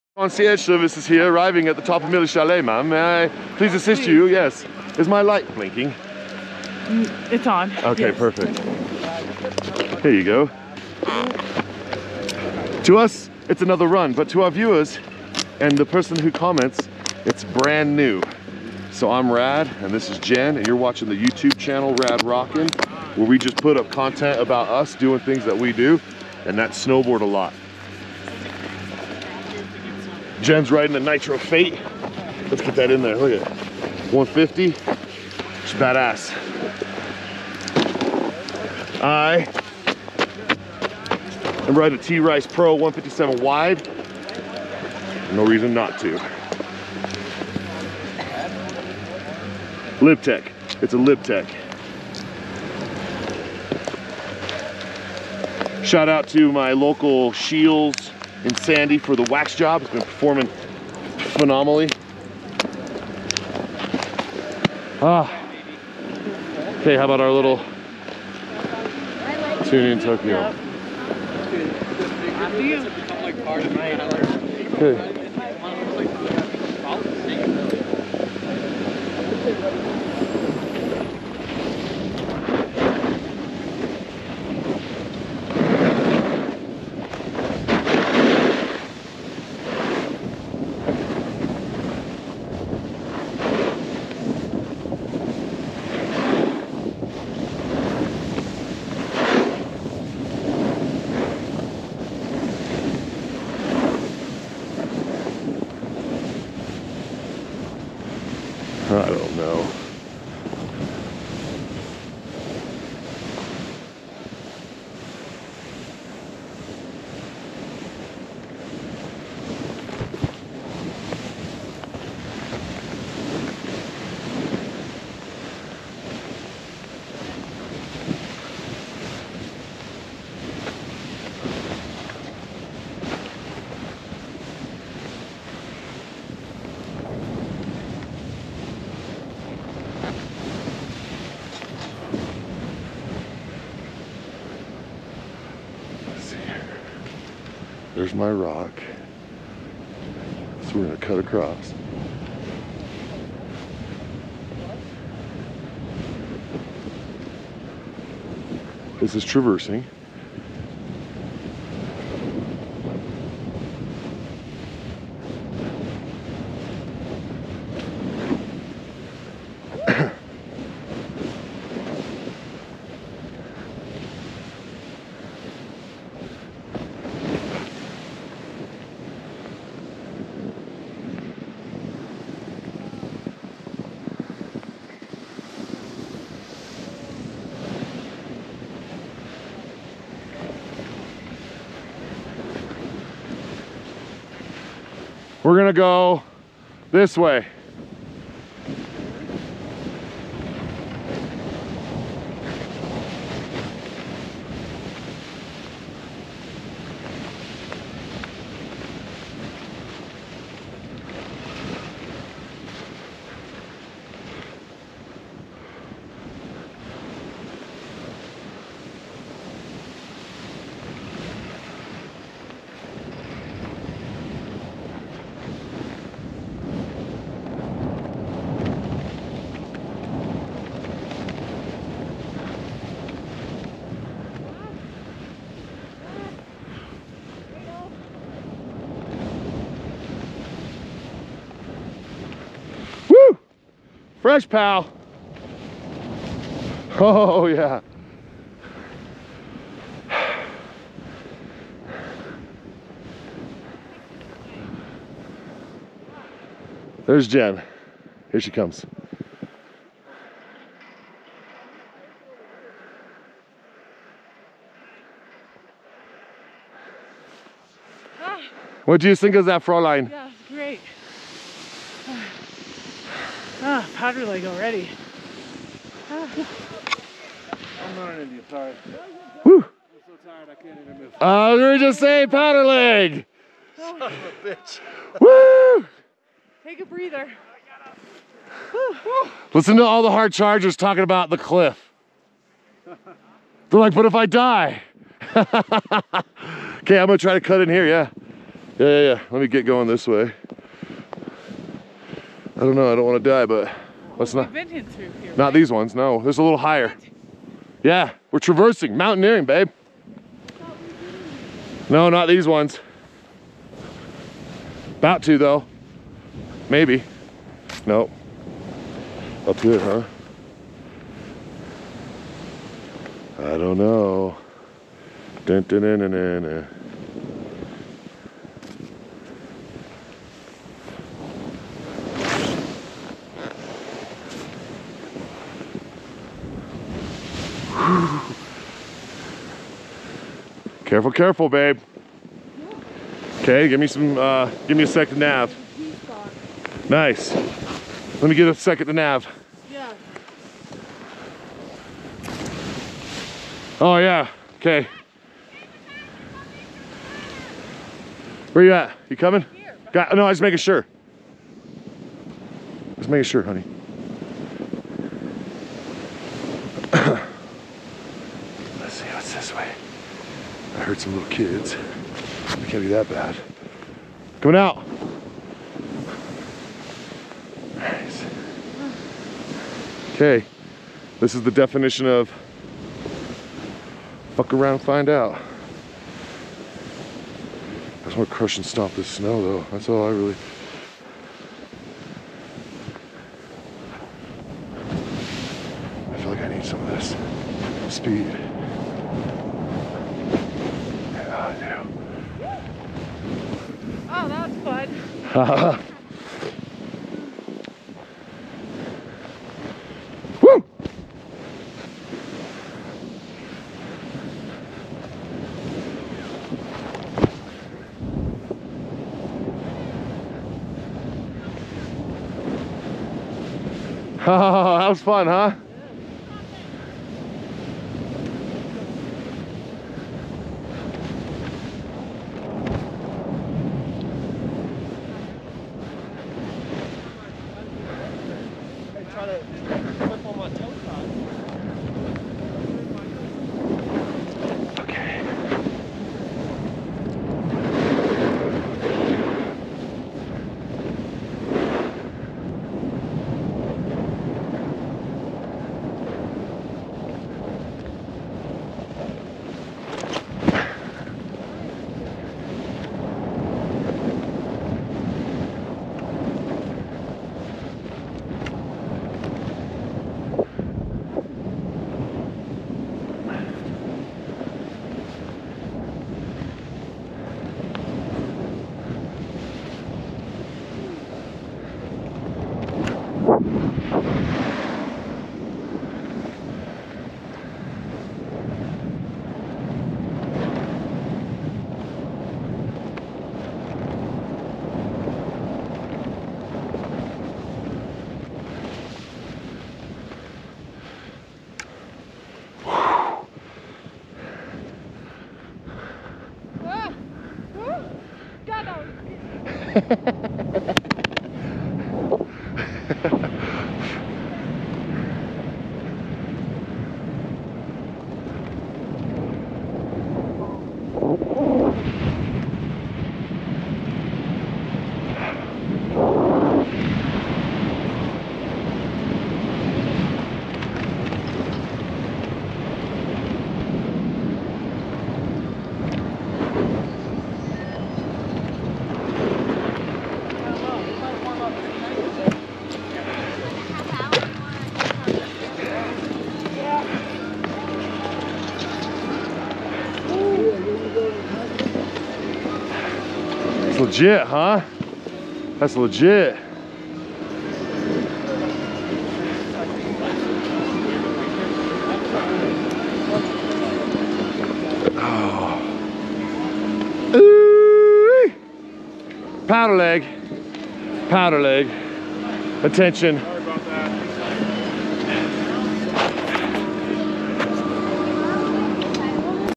2.22.24 Rad and Jenn Traverse little MILLY BRIGHTON SKI RESORT
2.22.24 Rad and Jenn Traverse little MILLY BRIGHTON SKI RESORT

Jenn and I explore Milly and her beautiful runs. we invade the trees and mountaineer our way to a FUN time.

always wear a helmet  ⛑️
rad